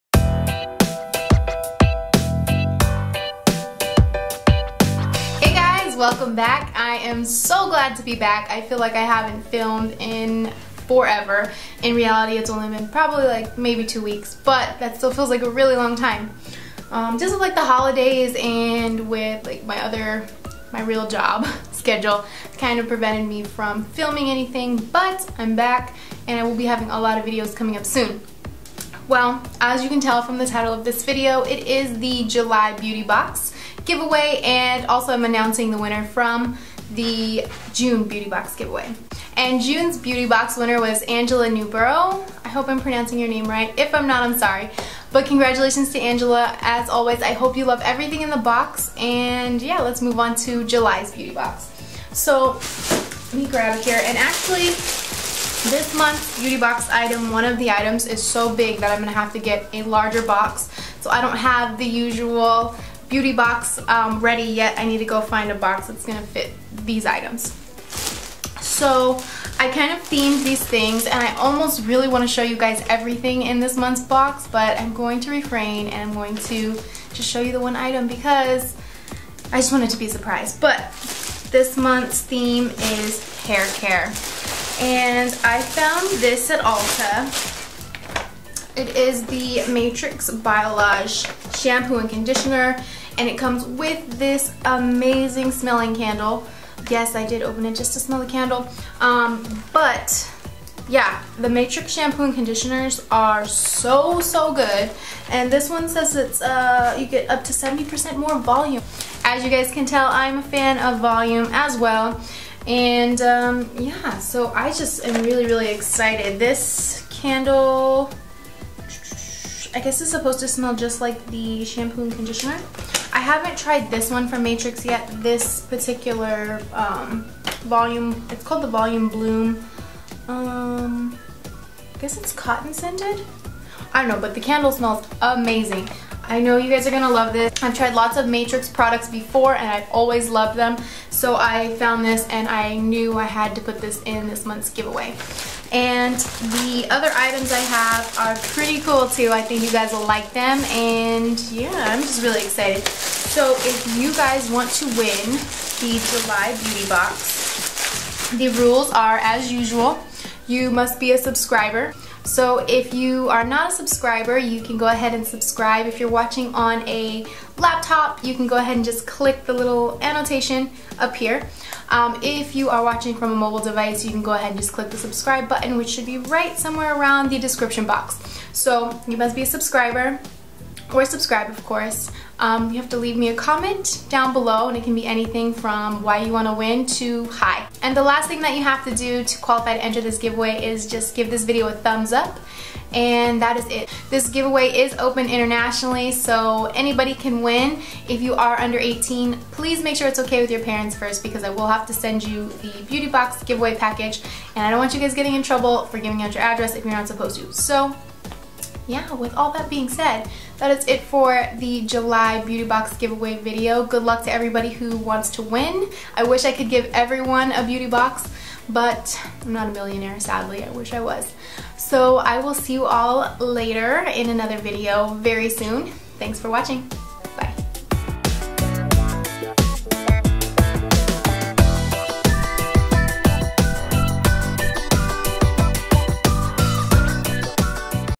July Beauty Box GIVE AWAY| Matrix Hair Care
Hey Guys this it the monthly beauty box give away. For July's box the winner will receive a box full of amazing hair care products and or styling tools. You never know every box is a surprise :)
Rules to enter:
2) Leave a comment (it can be anything)
This give away is open internationally. If you are under 18 please make sure you have your parents permission to enter. This give away is open until July 31, 2014